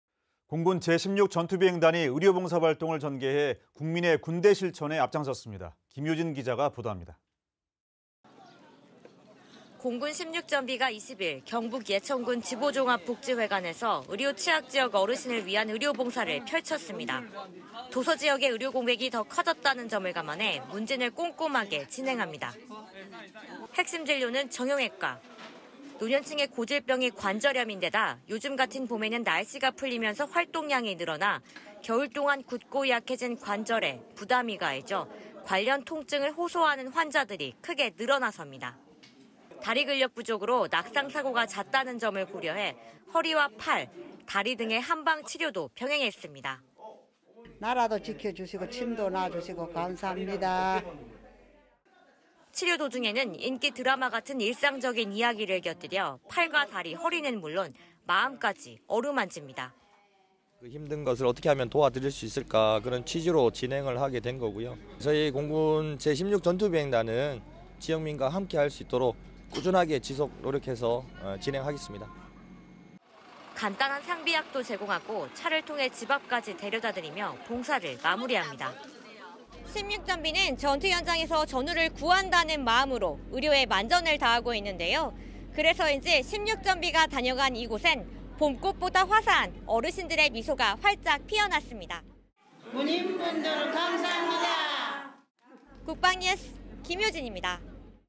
[2024 민·군 상생] 16전비, 무료 진료 봉사 [국방홍보원]
공군 제16전투비행단이 의료 봉사 활동을 전개해 ‘국민의 군대’ 실천에 앞장섰습니다.
김효진기자 가 보도합니다.

공군 16전비가 20일 경북 예천군 지보종합복지회관에서 의료 취약 지역 어르신을 위한 의료봉사를 펼쳤습니다.

도서지역의 의료공백이 더 커졌다는 점을 감안해 문진을 꼼꼼하게 진행합니다.

핵심진료는 정형외과.

노년층의 고질병이 관절염인데다 요즘같은 봄에는 날씨가 풀리면서 활동량이 늘어나 겨울 동안 굳고 약해진 관절에 부담이 가해져 관련 통증을 호소하는 환자들이 크게 늘어나섭니다.

다리 근력 부족으로 낙상사고가 잦다는 점을 고려해 허리와 팔, 다리 등에 한방치료도 병행했습니다.

[이금순(79세) / 경북 예천군]
: 군인이 나라도 지켜주고 침도 놓아주고 고맙습니다.

치료 도중에는 인기 드라마 같은 일상적인 이야기를 곁들여 팔과 다리 허리는 물론 마음까지 어루만집니다.

[심명성 대령 / 공군 16전비]
: (주민이) 힘들어하는 것을 어떻게 도와드릴 수 있을까 그런 취지에서 진행을 한 것이고요, 공군 제16전투비행단은 지역민과 함께할 수 있도록 꾸준하게 지속 노력해서 진행하겠습니다.

간단한 상비약도 제공하고 차를 통해 집 앞까지 데려다 드리며 봉사를 마무리합니다.

: 16전비는 전투 현장에서 소중한 전우를 구한다는 마음으로 어려움에 처한 국민을 위해 만전을 기하고 있는데요, 그래서 인지 16전비가 다녀간 이곳엔 봄꽃보다 아름다운 어르신들의 화사한 미소가 활짝 피어났습니다.

[군인분들 감사합니다]

국방뉴스 김효진 입니다.